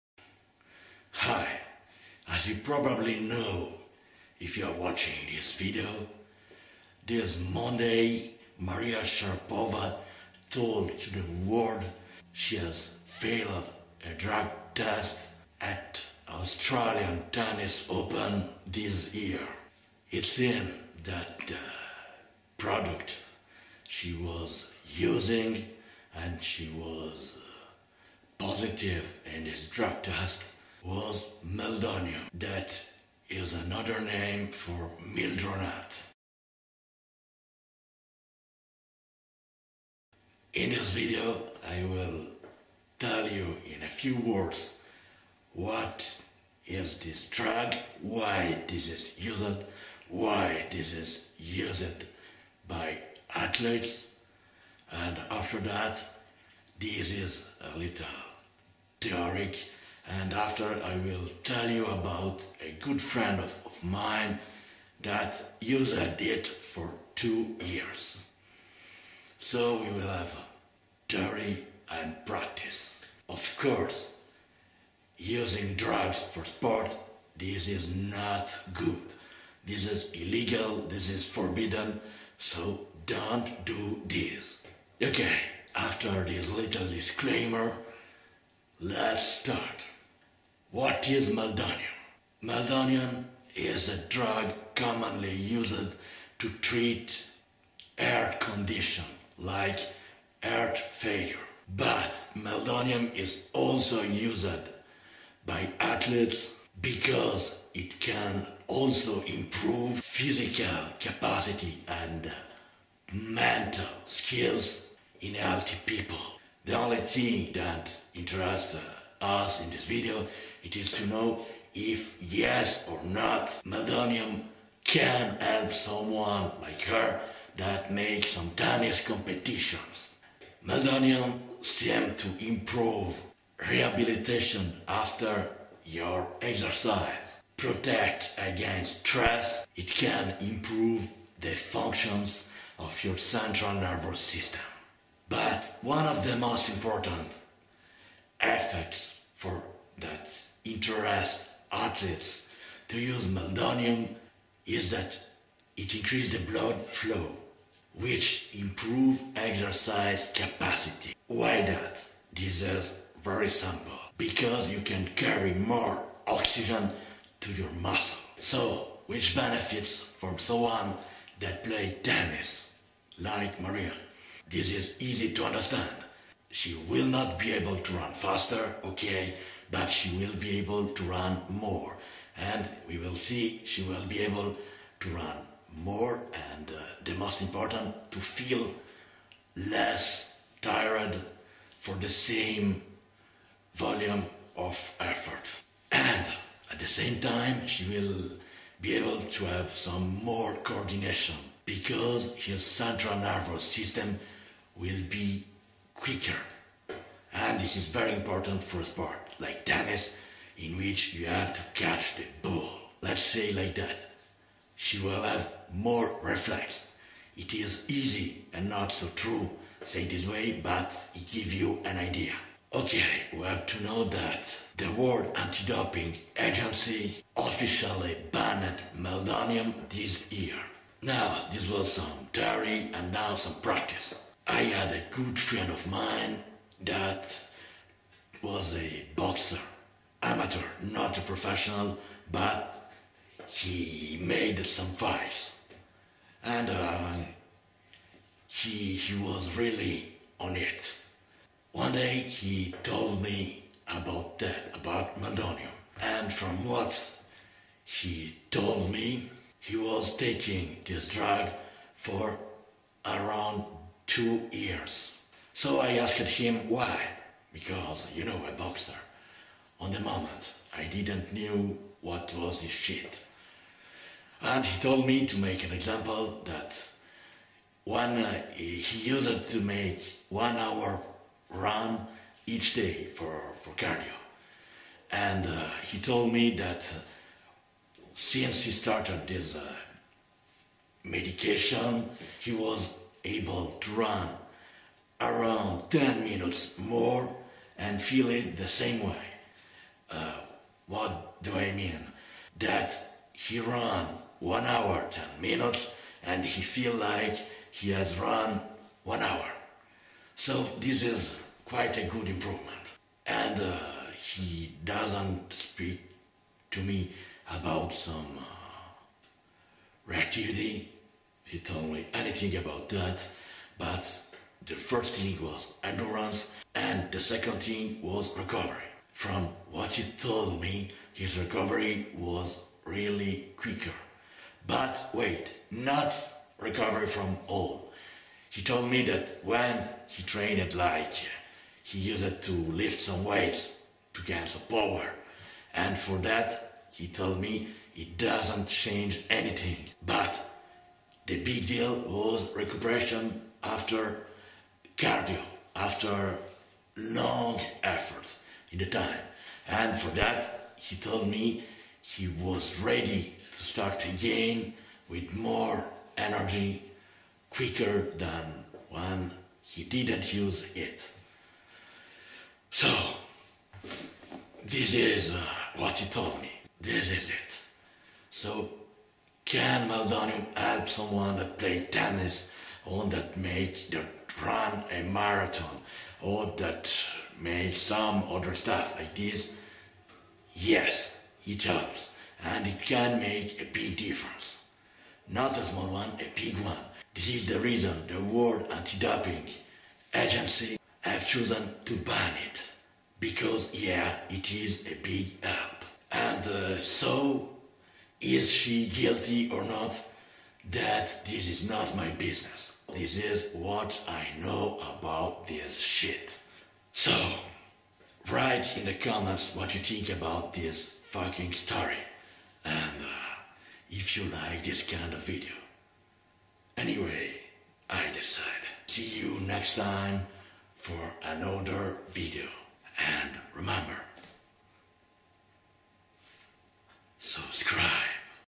Meldonium drug and his usage by athletes
Old video of NinjaTyler talking about Meldonium and why is it so popular among athletes.